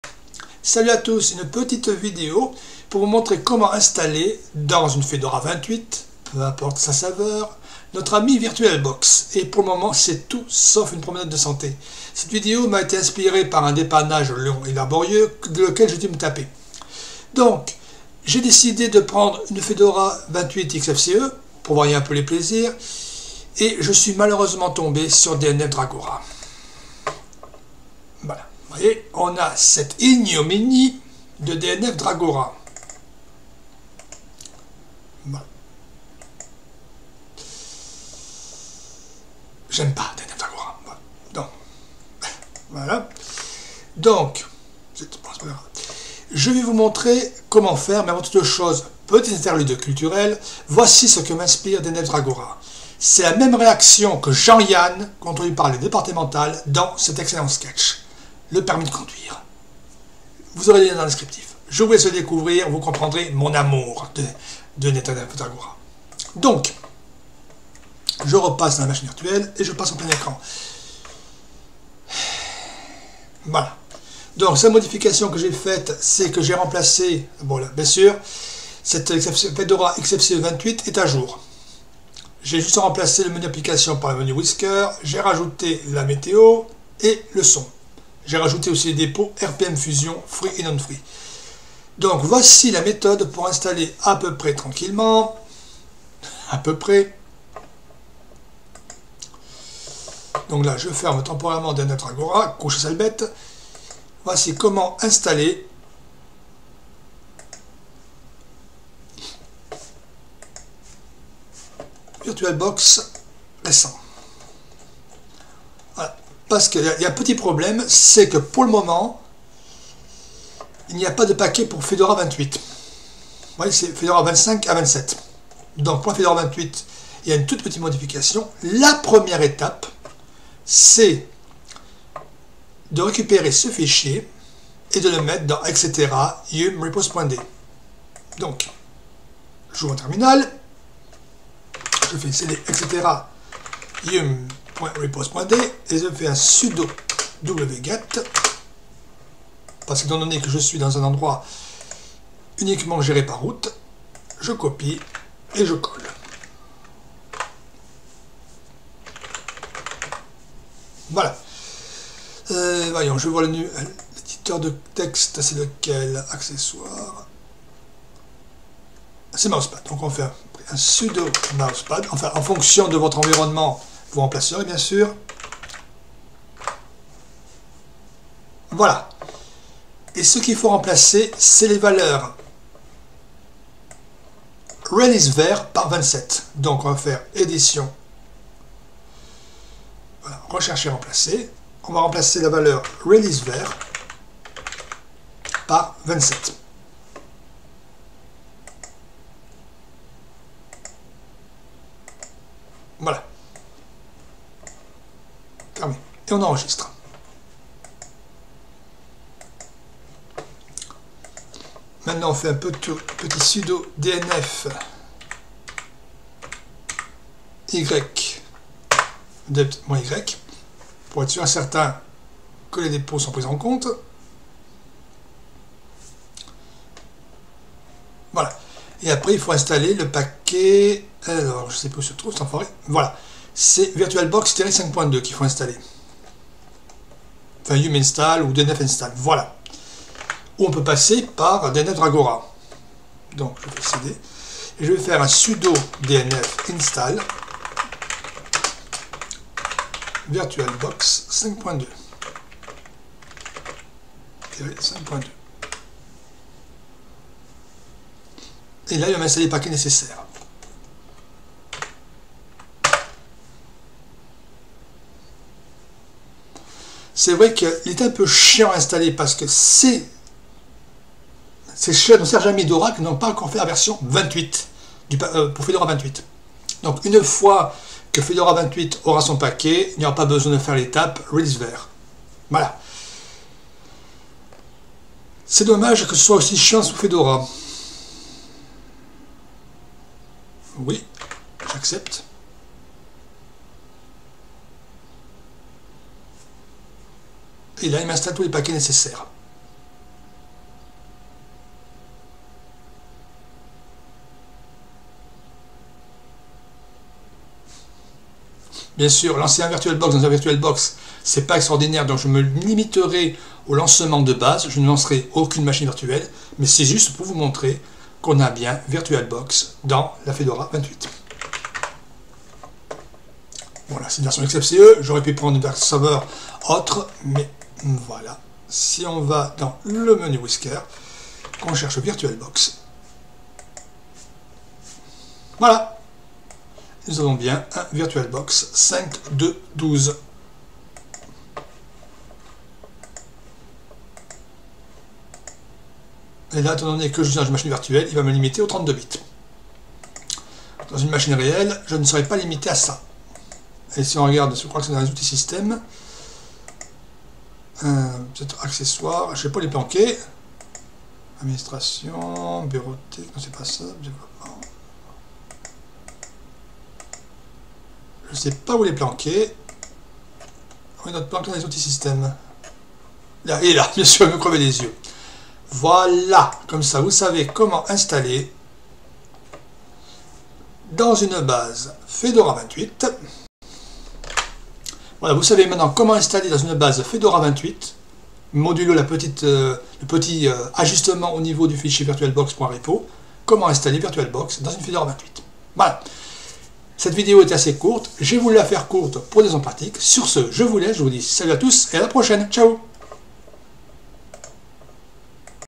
[Vidéo obsolète] Installer VirtualBox dans Fedora 28... Quelle galère :)
Une vidéo enregistrée suite à un dépannage long et laborieux.

Note : la modification du fichier virtualbox.repo sera inutile avec la sortie de paquet pour Fedora 28.

Horodatage :

00:28 : on se tape cette ignominie inergonomique qu'est DNF Dragora :(
00:52 : un sketch des années 1960 avec Jean Yanne qui passe son permis de conduire...
01:17 : un tour rapide des modifications de la Fedora 28 Xfce qui sert de base ici
01:54 : la documentation de VirtualBox pour l'installation
02:30 : on récupère le fichier virtualbox.repo
02:58 : la modification à faire le temps qu'Oracle propose des paquets dédiés...
04:06 : on installe Virtualbox 5.2...
04:21 : 17 paquets à installer ?
05:35 : et voila notre ami VirtualBox est installé...
06:11 : il est planqué dans le sous-menu système :)
06:20 : conclusion